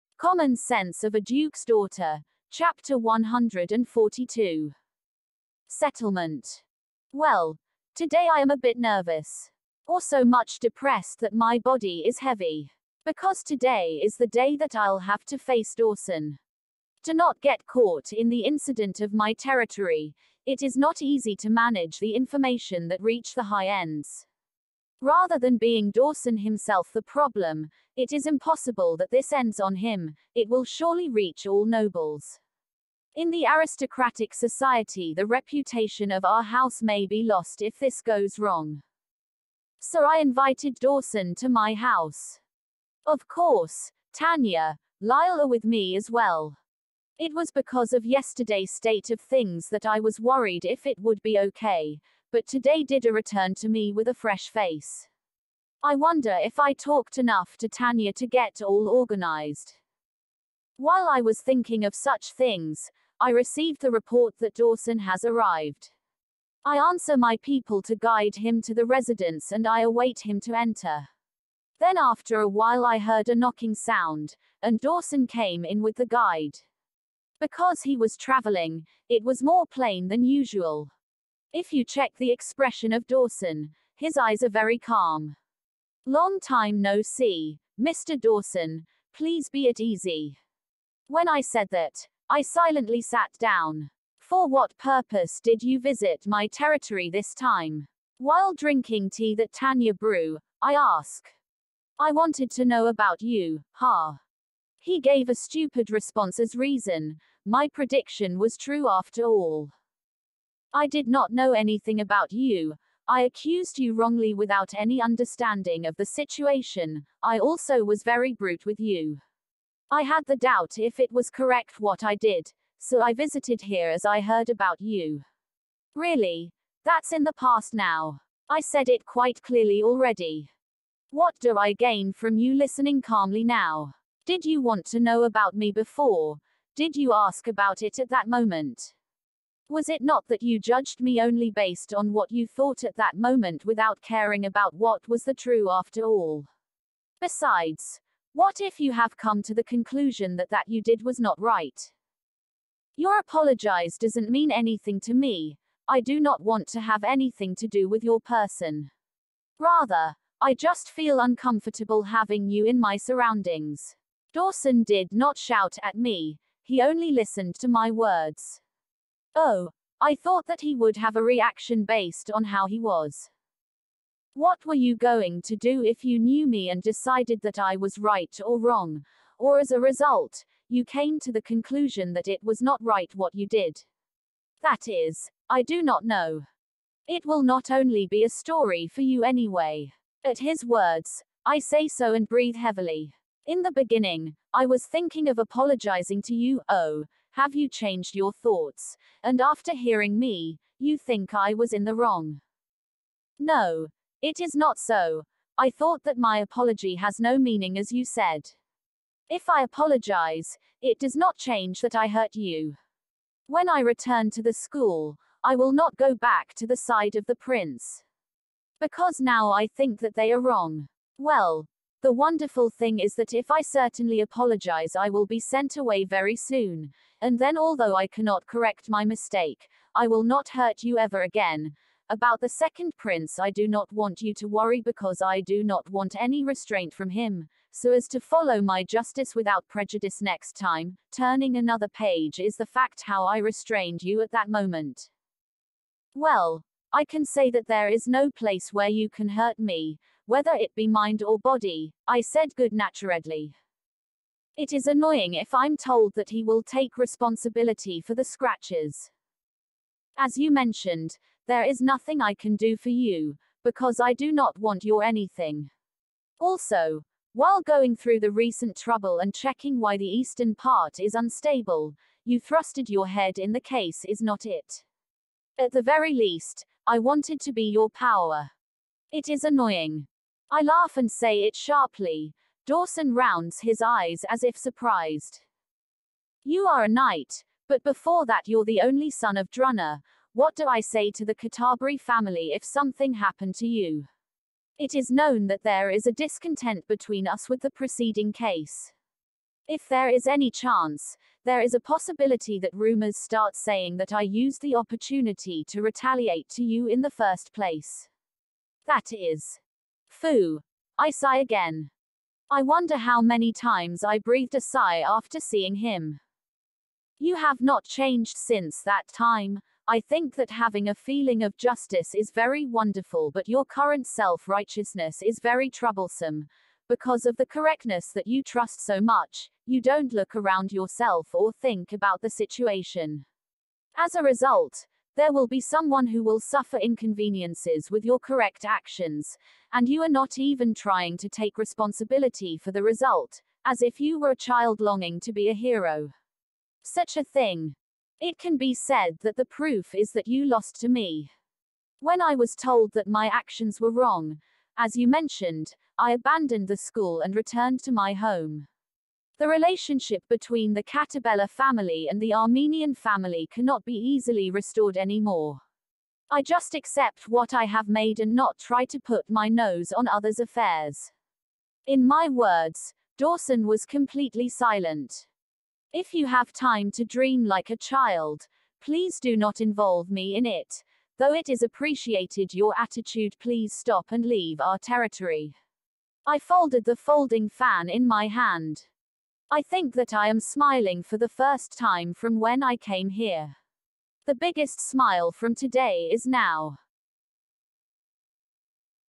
Common Sense of a Dukes Daughter (Koushaku Reijou no Tashinami) Chapter 142 - Settlement - Eng Audio
Summary
When a young woman is killed in a traffic accident on her way home from work at a tax bureau, she suddenly finds herself transported to the world of the otome game she was playing the night before – but instead of the heroine, she’s been reincarnated as the villainess!

Using her knowledge of the game, “Iris” manages to avert personal disaster and decides to rebuild her life with her modern-day economic know-how. This is one mean girl who isn’t going to let her perceived reputation stop her from being a heroine!
Associated Names
Accomplishments of the Duke’s Daughter
Koushaku Reijou no Tashinami
เกมรักศักดิ์ศรีบุตรีดยุก
公爵令嬢の嗜み
公爵千金的爱好
公爵家千金大小姐的爱好